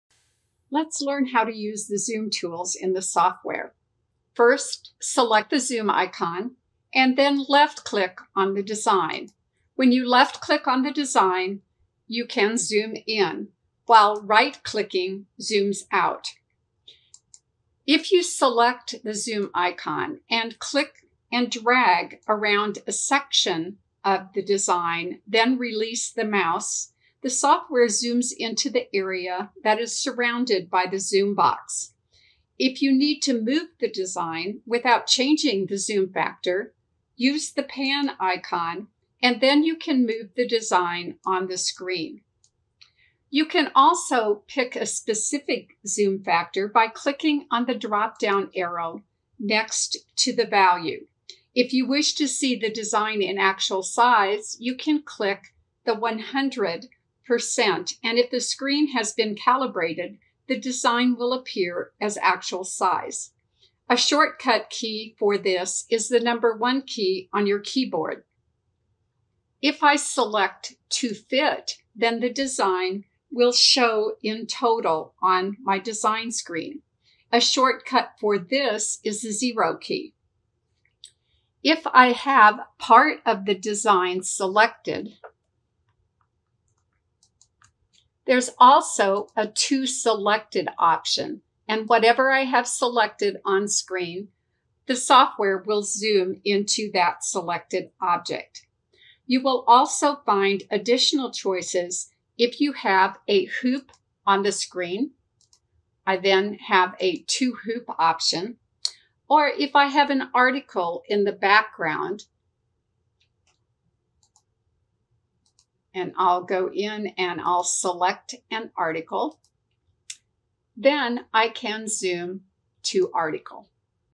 1.08 Master BERNINA Embroidery Software 8 – Getting Started – Zoom Toolbar
Discover how to properly use the Zoom Toolbar in BERNINA Embroidery Software 8.